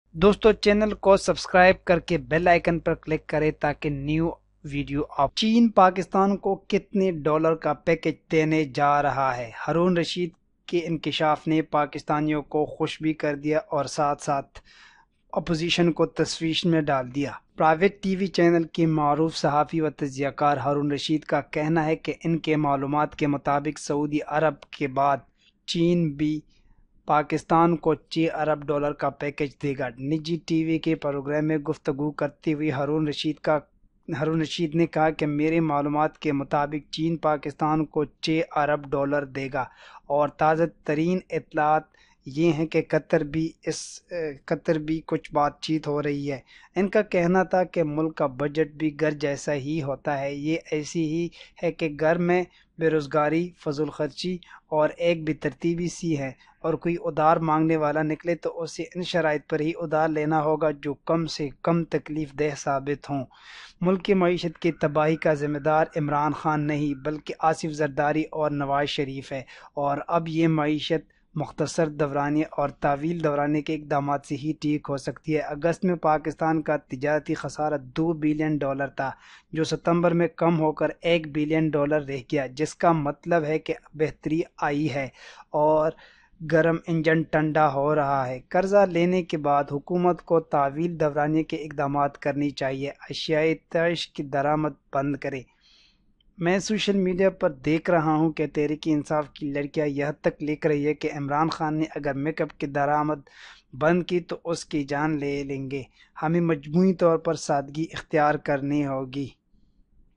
China will give pakistan 6 billion say Haroon Rashid
China will give pakistan 6 billion say Haroon Rashid.Senior anchor person Haroon Rasheed says China will also give 6 billion dollar to pakistan after saudi arabia he says further more that Qatar is also going to give some loan to pakistan